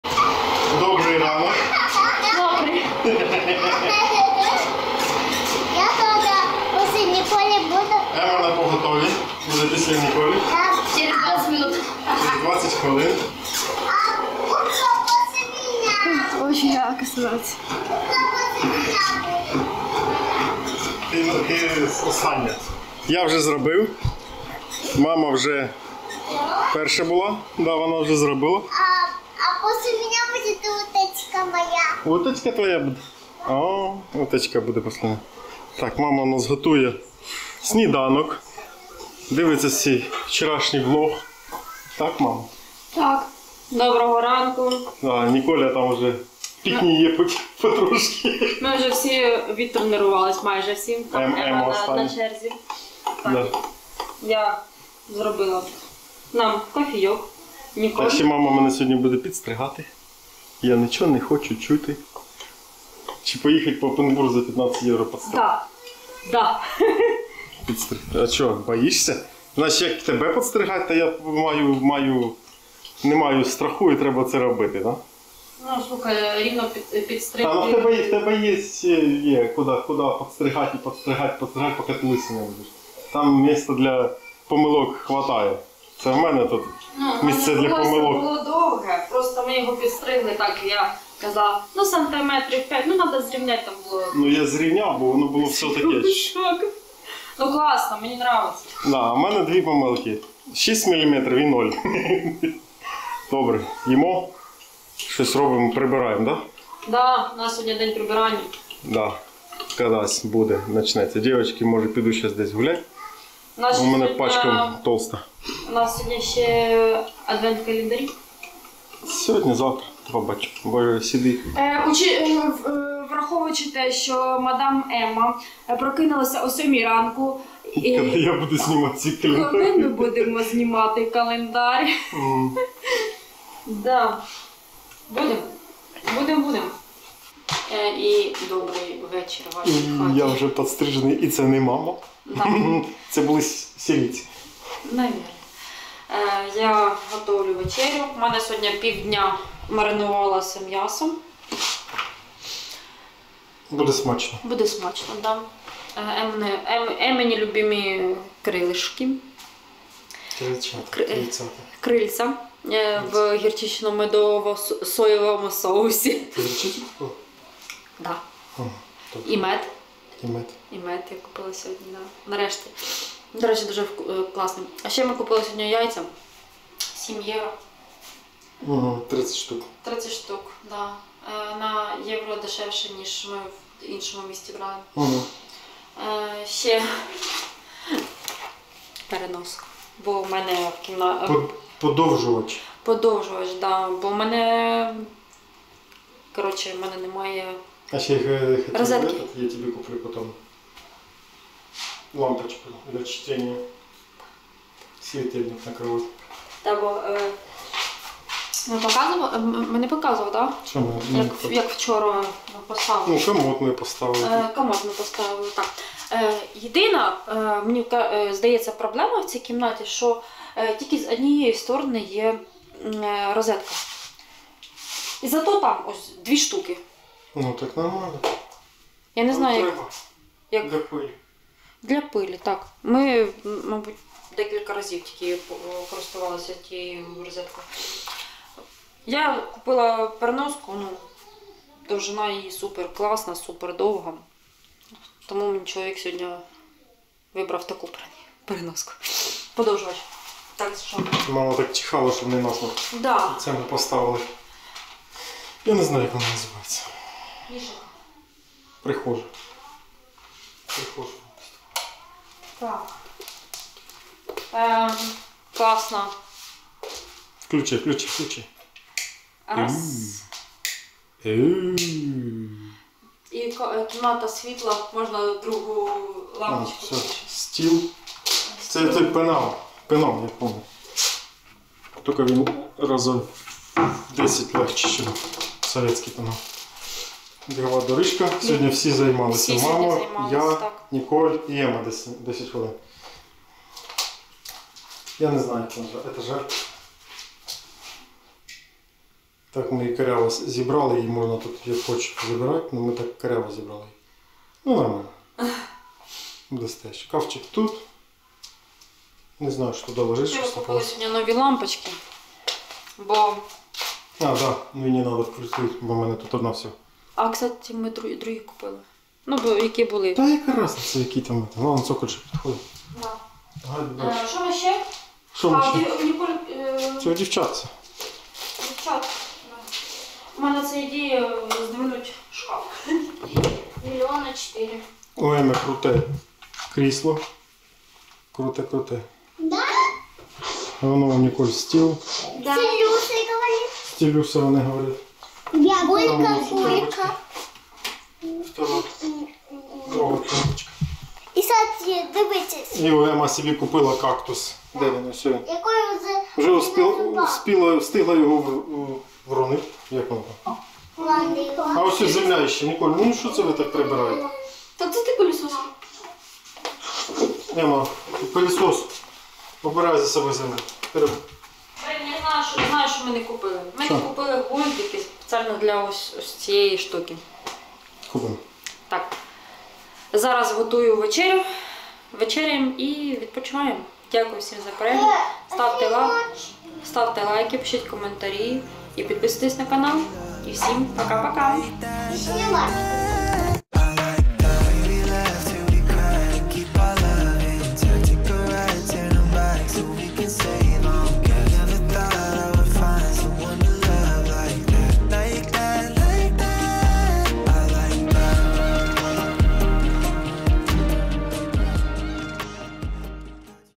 Тренувалися. Готую смачну вечерю. 12.11.2022
Далі більше.

Влог о  новому житті.
Vlog about new life